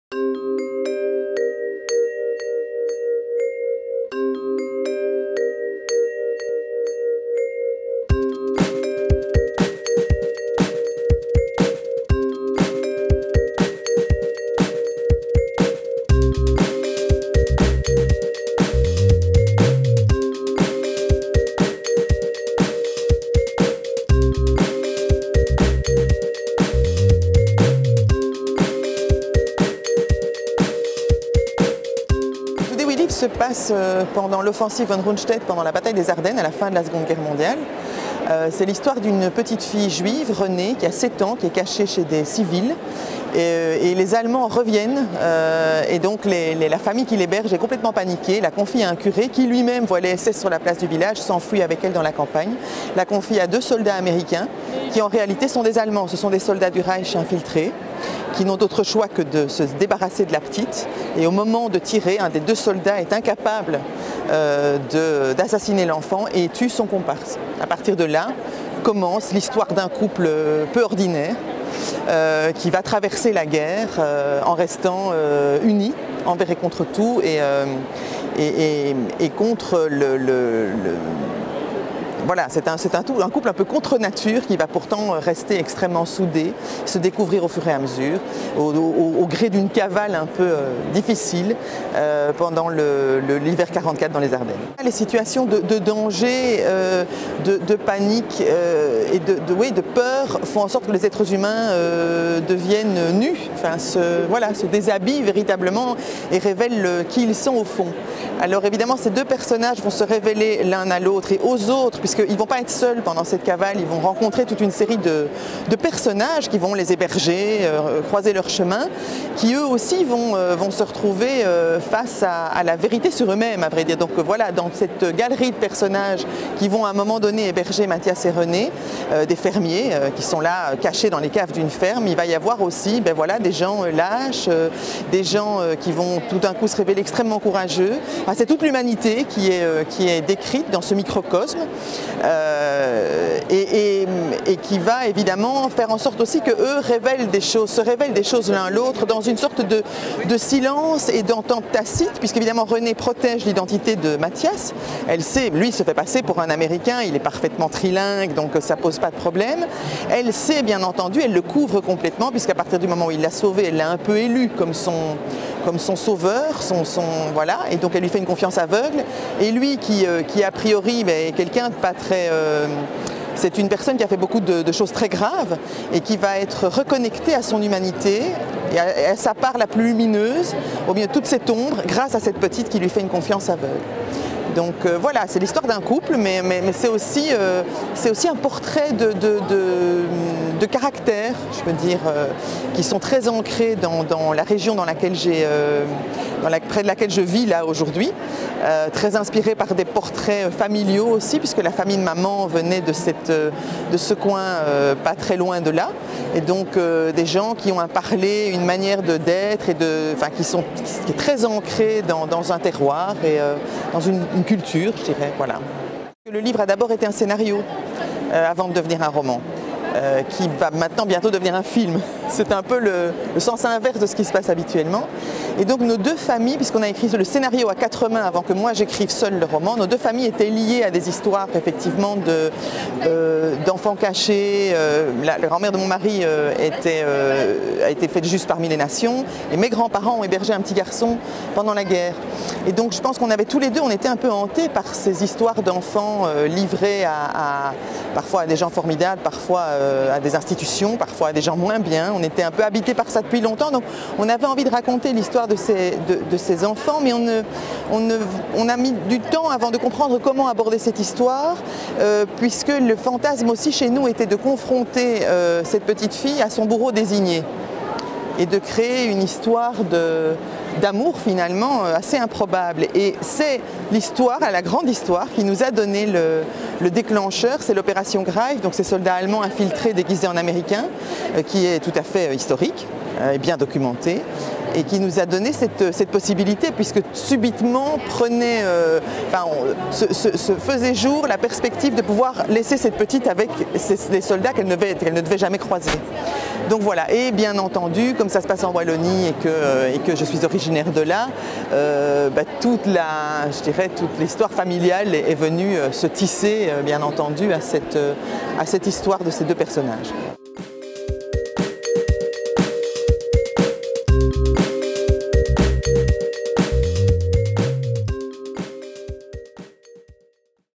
Emmanuelle Pirotte - Today we live
Emmanuelle Pirotte vous présente son ouvrage "Toady we live" aux éditions Cherche Midi à l'occasion de Livre Paris 2016.